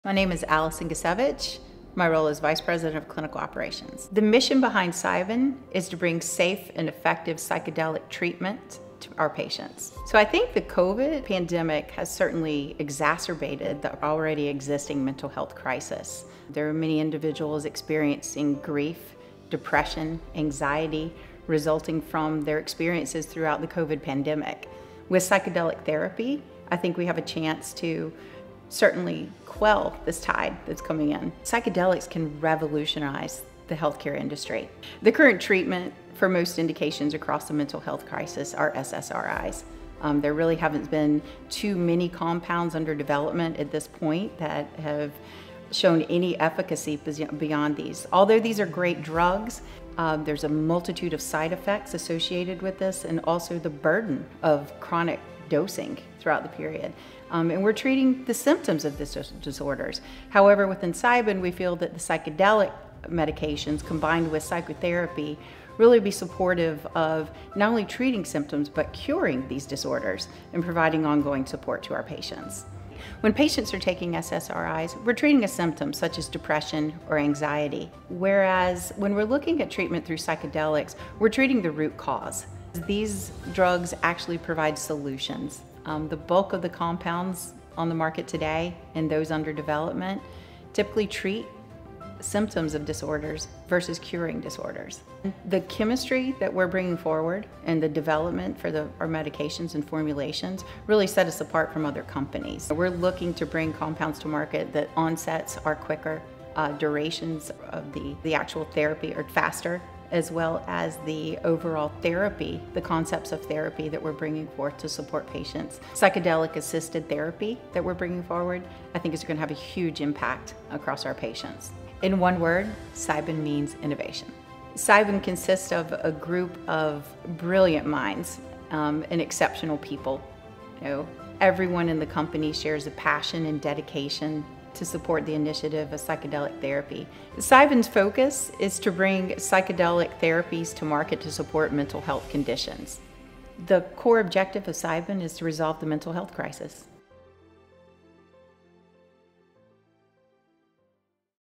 Bringing Safe & Effective Psychedelic Treatments To Our Patients | Allison House-Gecewicz
Allison House-Gecewicz - VP Clinical Operations shares how Cybin's mission is to bring safe and effective psychedelic treatment to their patients.

Cybin plans on bringing forward psychedelic compounds that have a quicker onset of delivery, shorter time durations, and a tailored treatment approach of psychedelic-assisted psychotherapy.

NYSE AMERICAN: CYBN
NEO: CYBN

Interested in learning more about Cybin? Here's where to follow Cybin's latest news: